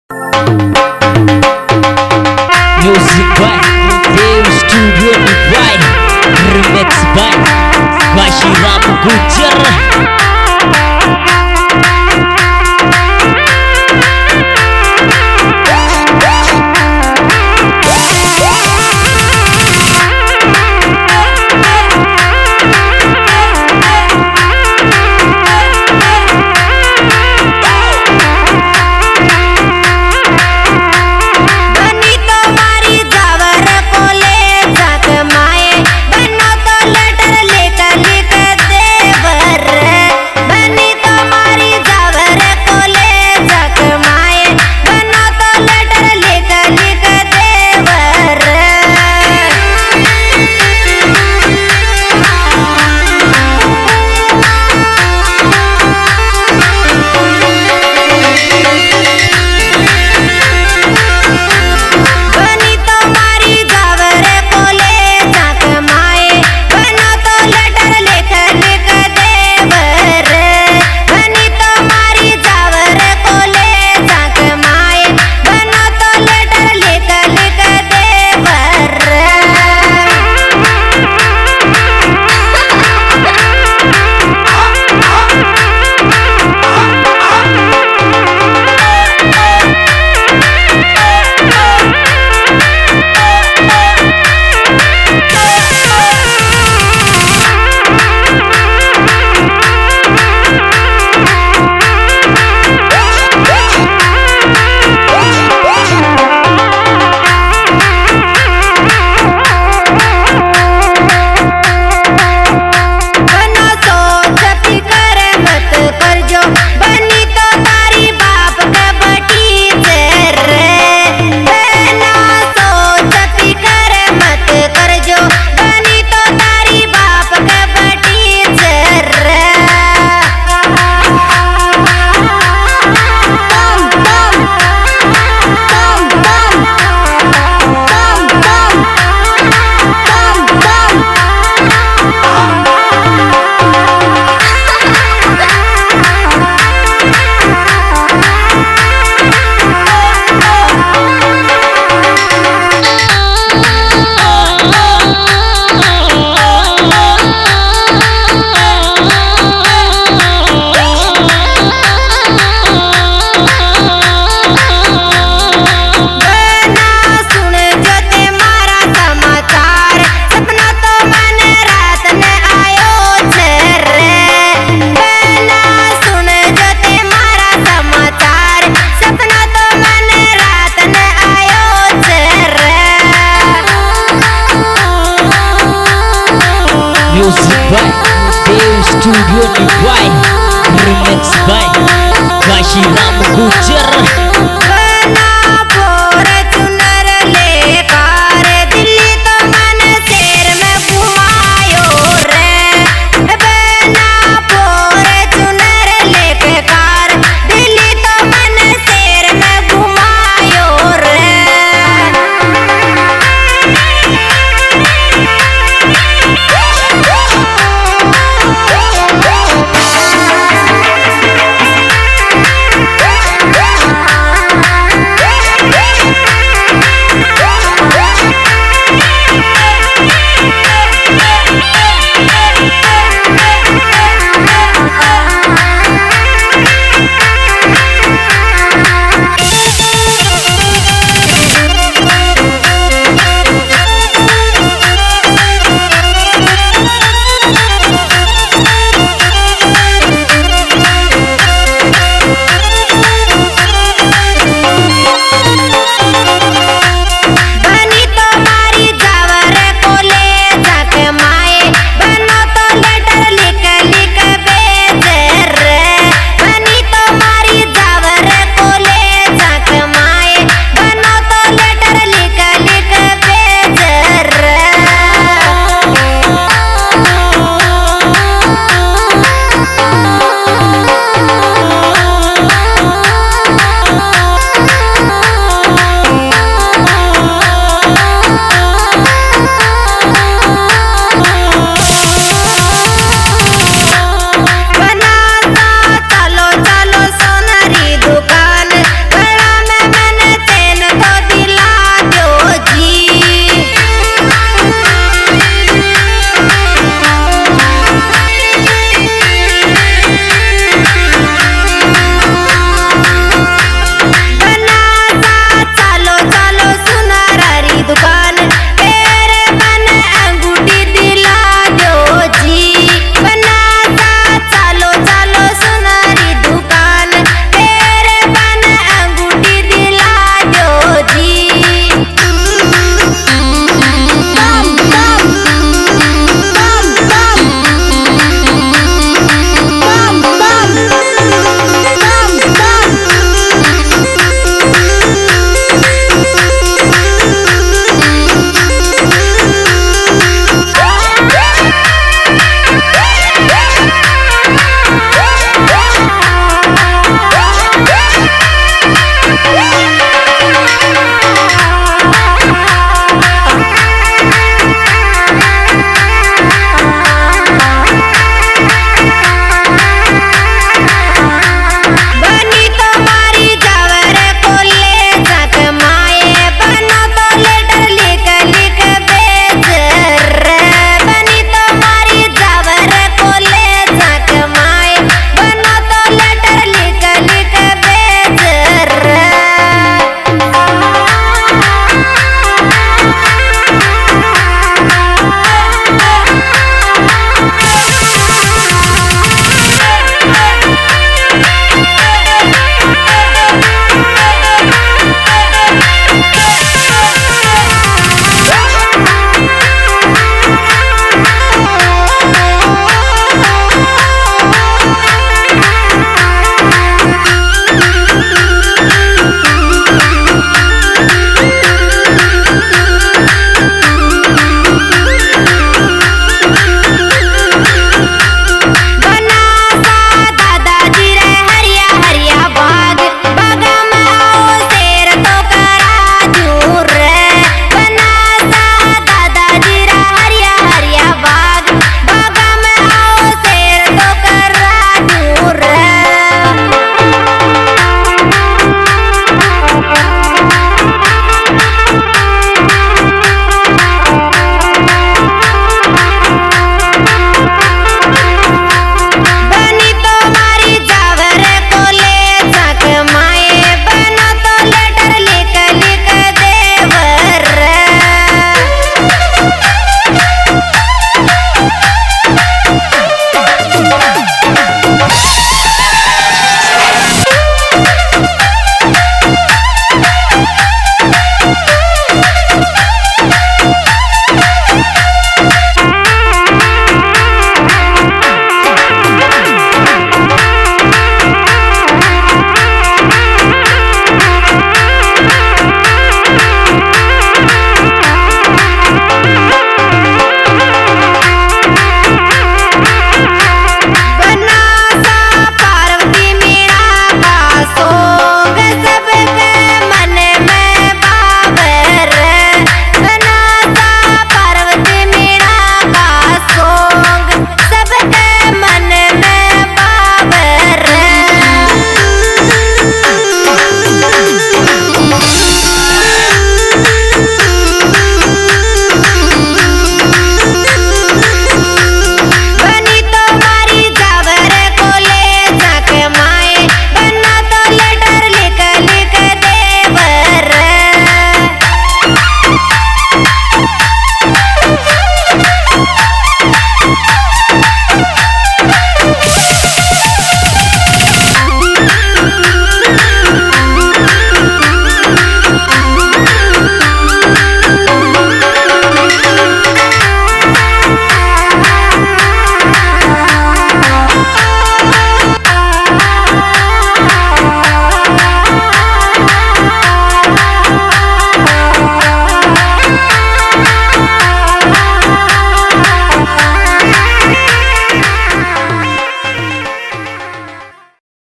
राजस्थानी भजन 🎸 सिंगर, सियाराम मीणा, व पार्वती मीणा की आवाज़ में शानदार बन्ना बन्नी विवाह गीत
New Song 2020 बन्ना बन्नी विवाह गीत

Singer - Siyaram Meena & Parwati